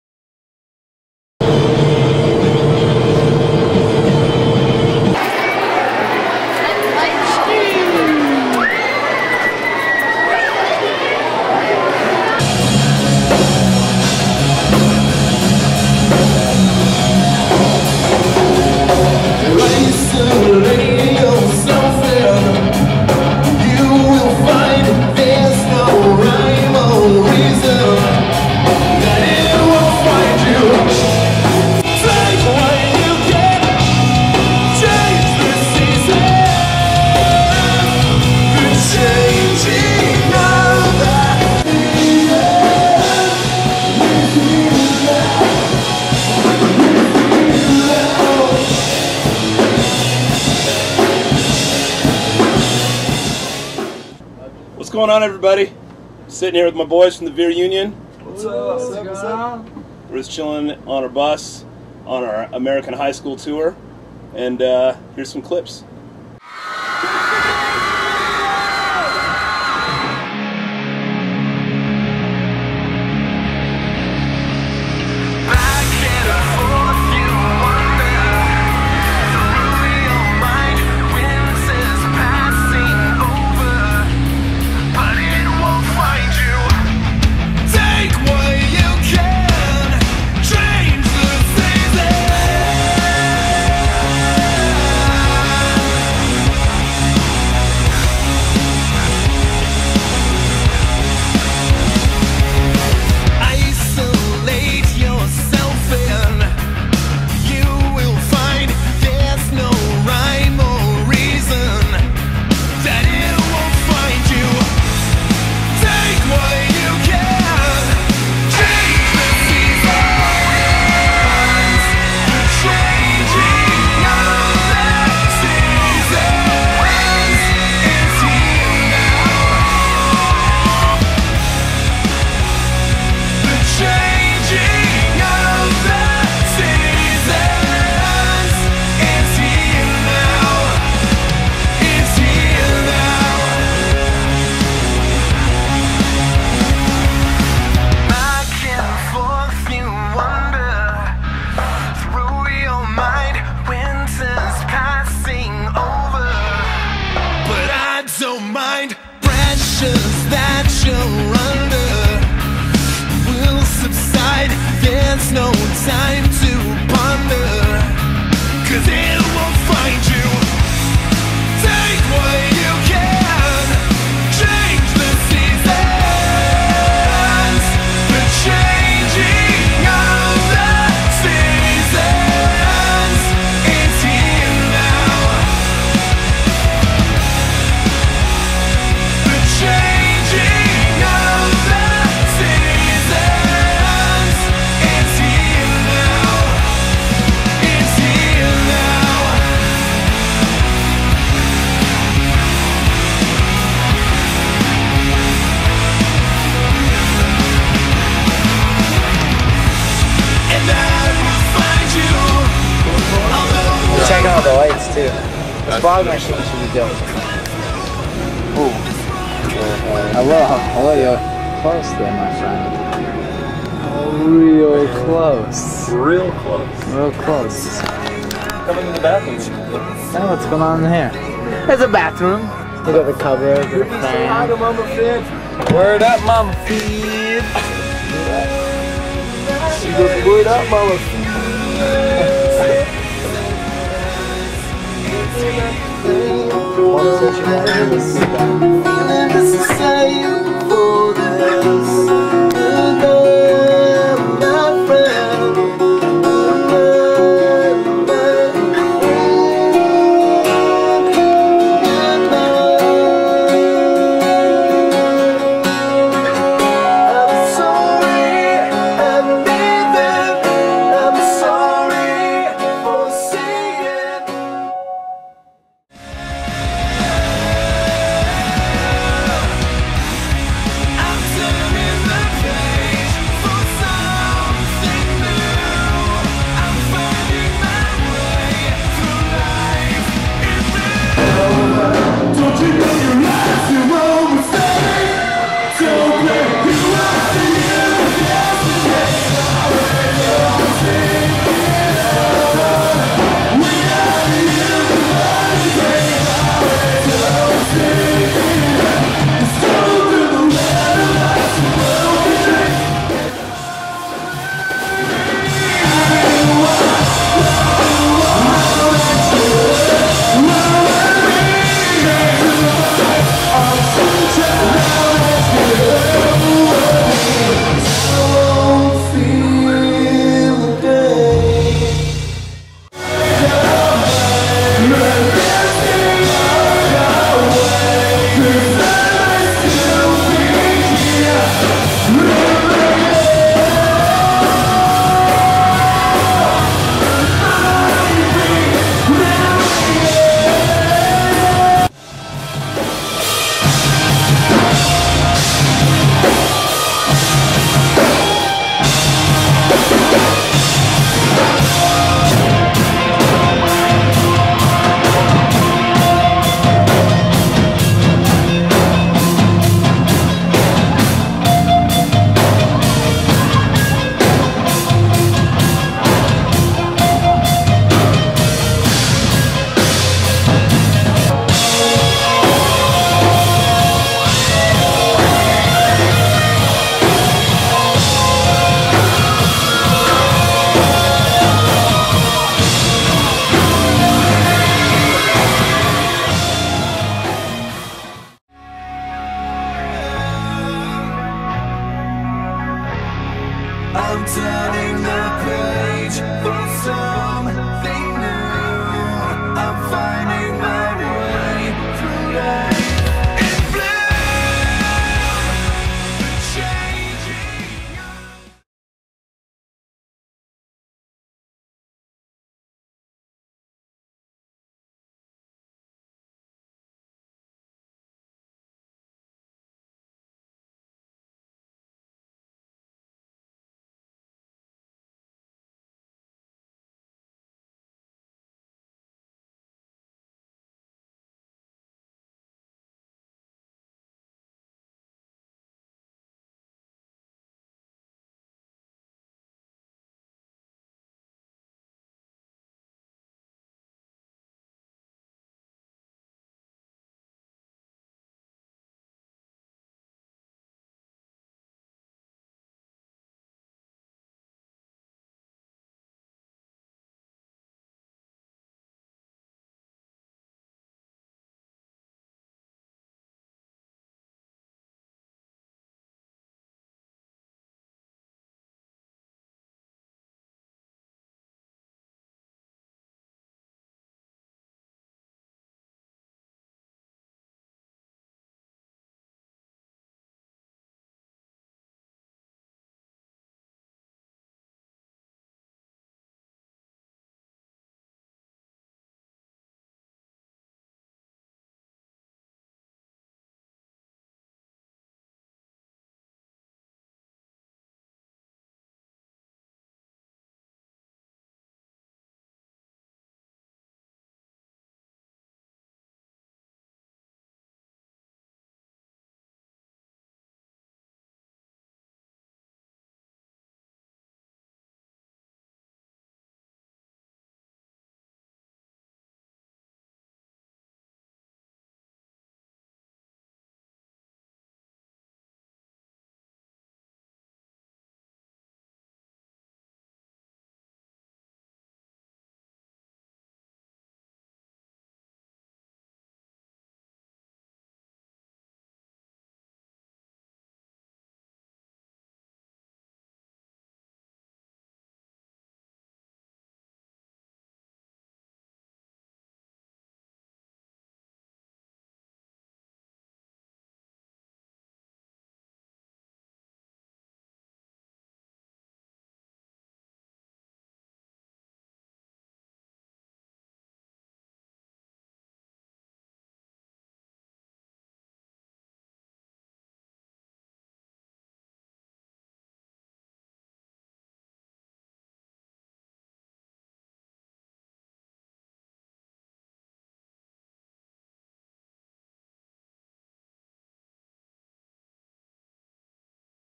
The Veer Union High School Tour... Vol. 1
Footage from TVU's US high school tour... 2008. volume 1